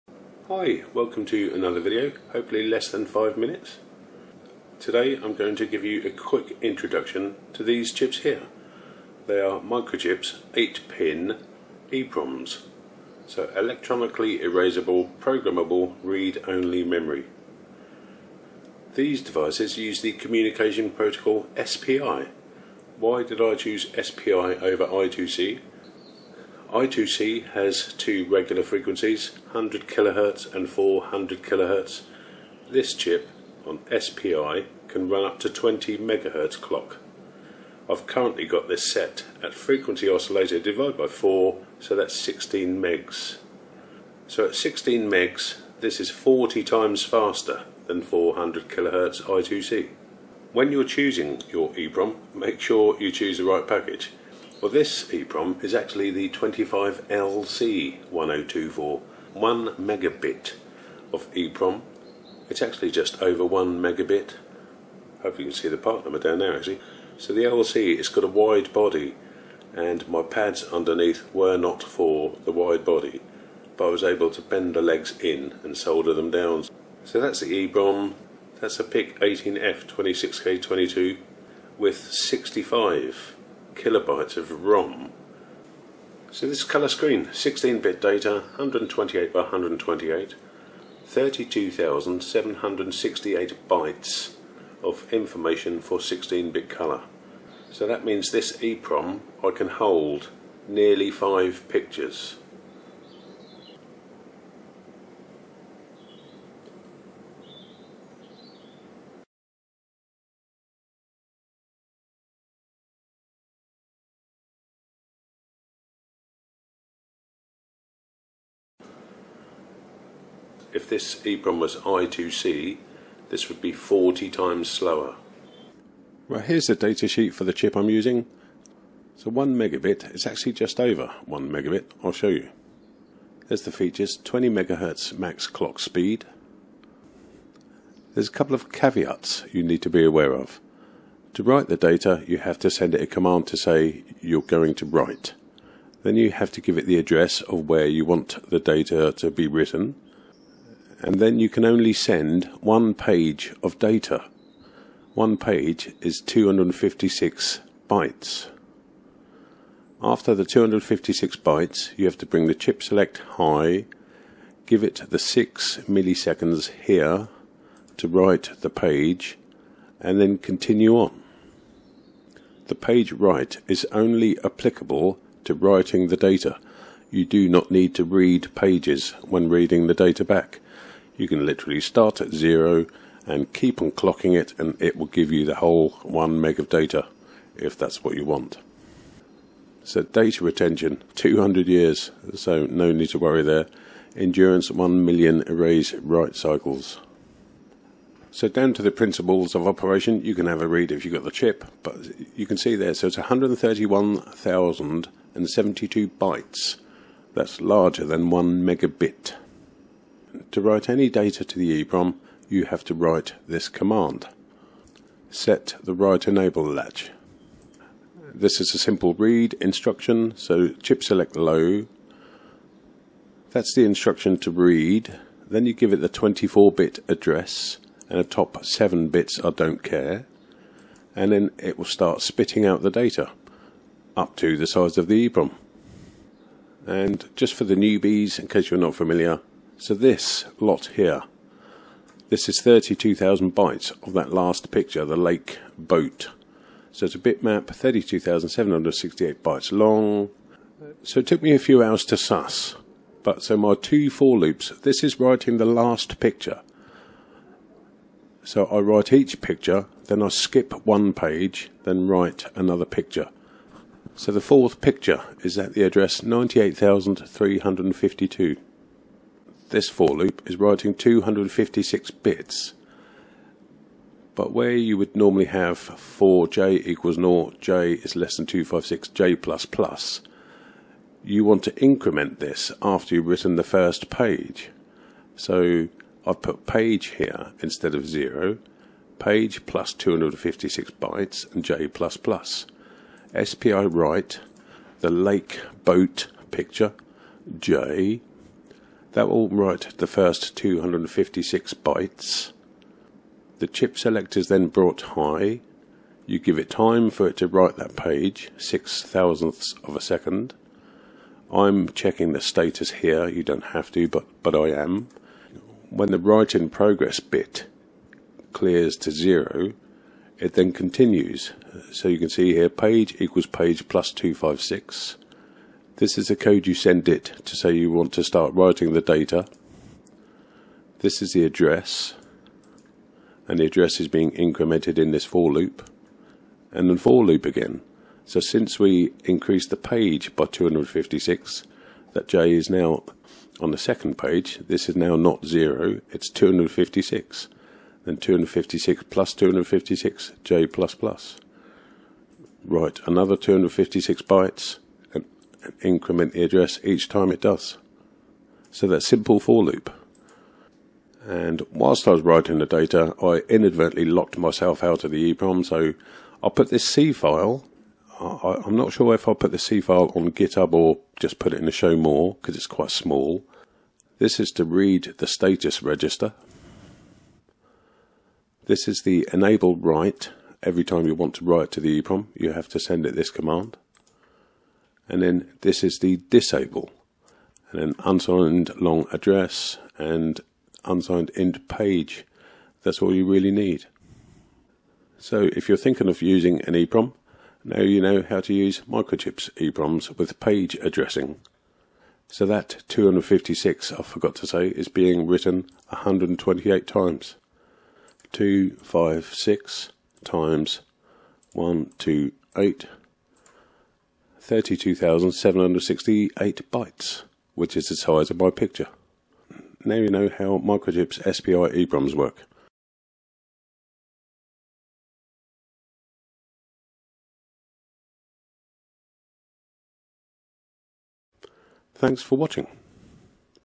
Microchip's EEPROMs, How to Program in C, 25LC1024 1 MbIT
Hi, a quick look at how to program a tonne of data onto a Microchip EEPROM with 2 simple 'for loops'.
25LC1024, 25AA1024 1 MEGABIT